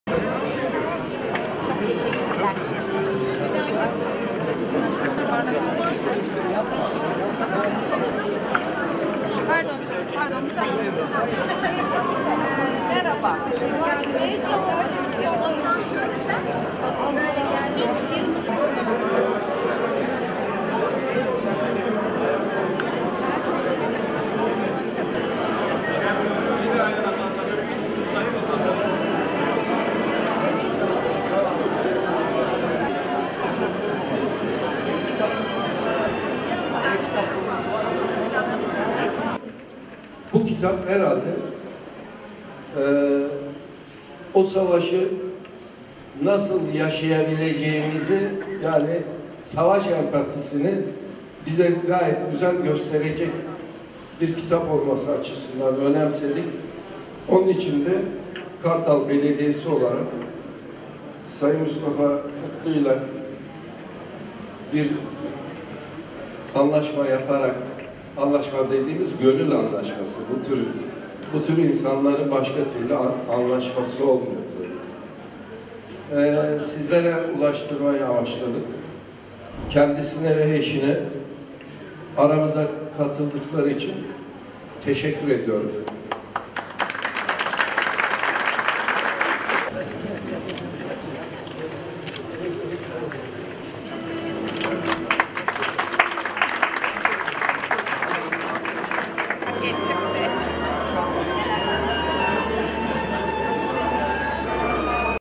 BAŞKAN ALTINOK ÖZ, GAZETECİ MUSTAFA MUTLU’NUN İMZA GÜNÜNE KATILDI
Kartal Belediye Başkanı Op. Dr. Altınok Öz’ün öncülüğünde gerçekleşen ve kültür sanat alanında verilen hizmet kapsamında gazeteci- yazar Mustafa Mutlu’nun “74. Ferman” adlı kitabı için Kartal Belediyesi Hizmet Binasında imza günü düzenlendi.
Bu video Kartal Belediyesi Basın Yayın ve Halkla İlişkiler Müdürlüğü tarafından yayına hazırlanmıştır.